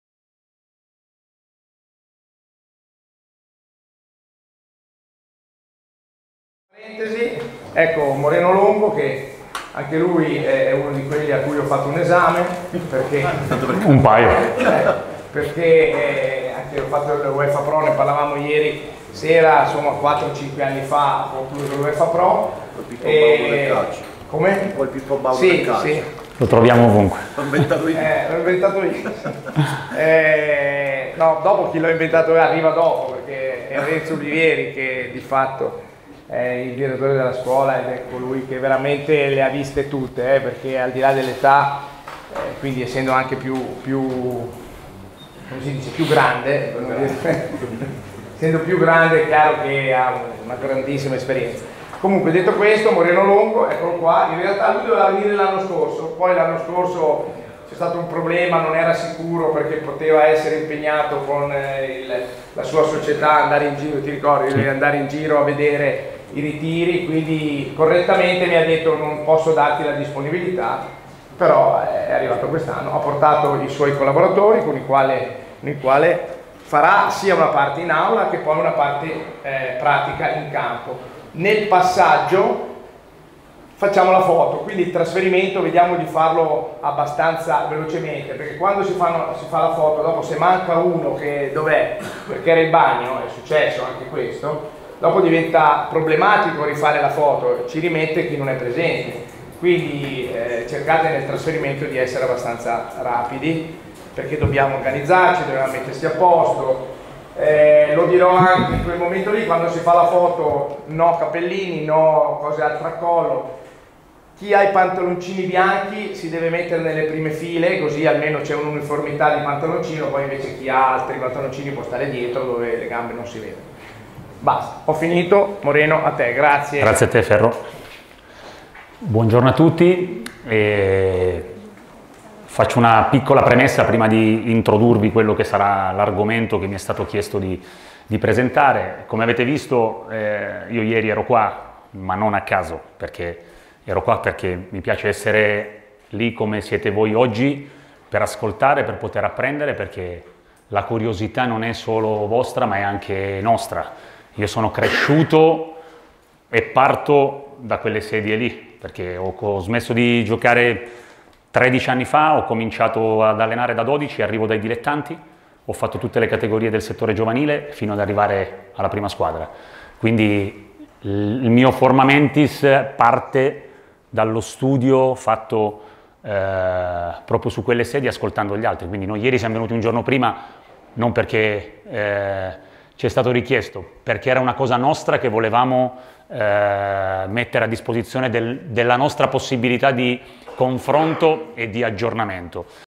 'La trasformazione dei sistemi di gioco' a cura di Moreno Longo
"La trasformazione dei sistemi di gioco", lezione del Master 2019 de Il Nuovo Calcio tenuta da Moreno Longo.